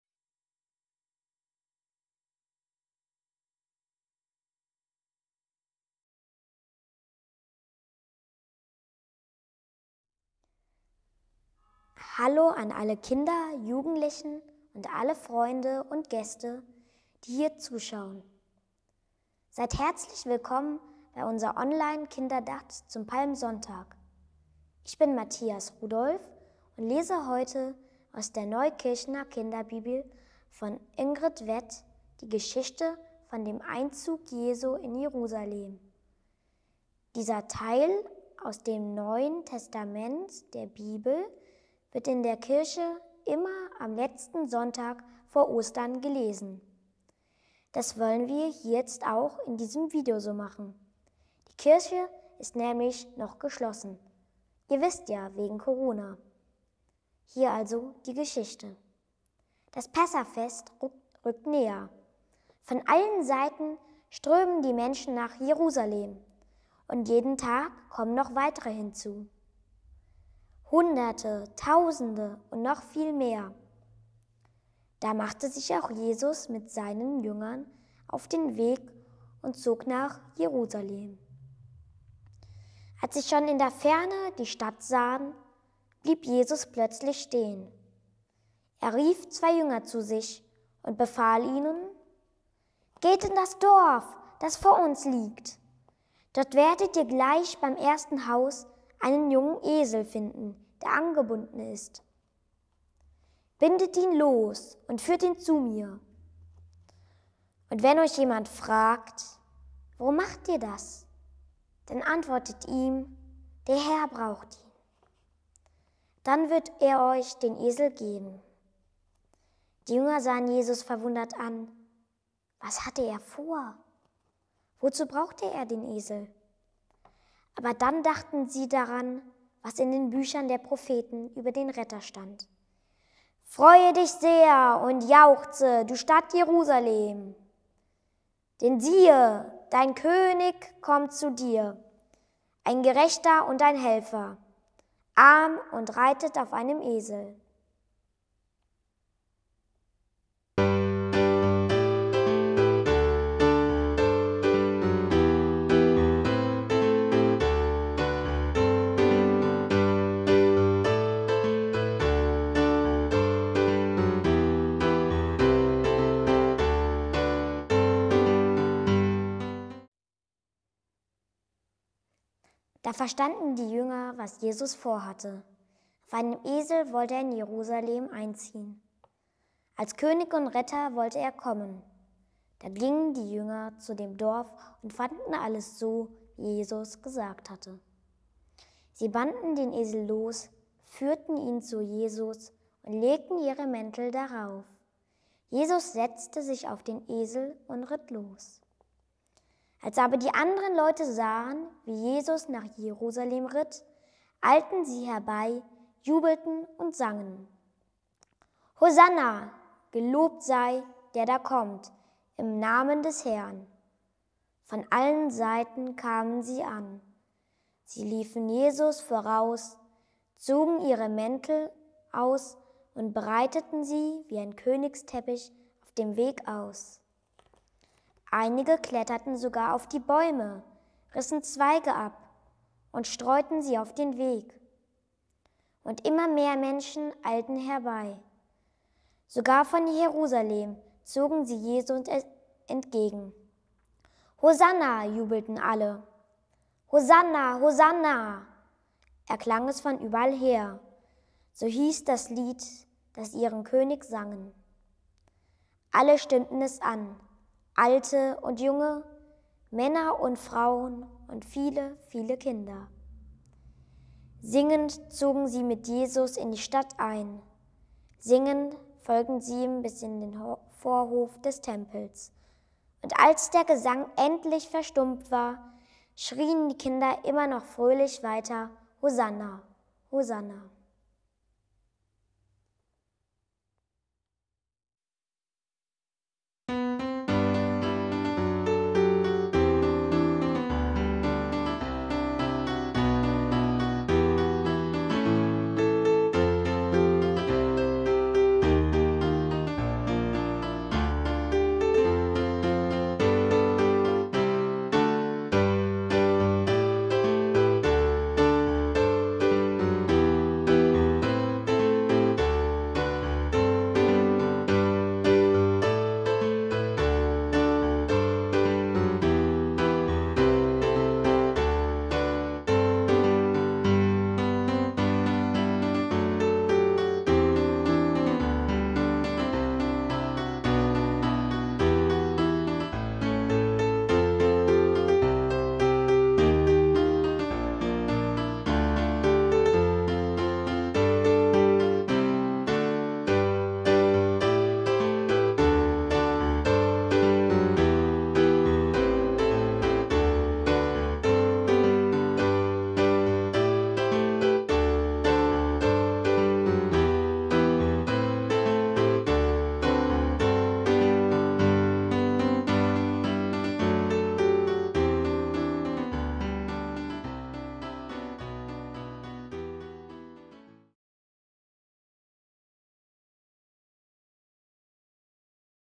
Die Geschichte des Einzugs Jesu in Jerusalem
Liebe Kinder und alle Gäste, die hier zuschauen: Herzlich willkommen zur Andacht für euch zum Palmsonntag. Matias Rudolph hat in diesem Video die Moderation übernommen und erzählt euch die Geschichte vom Einzug Jesu in Jerusalem aus der Neukirchener Kinderbibel von Irmgard Weth. Macht's gut und bleibt gesund!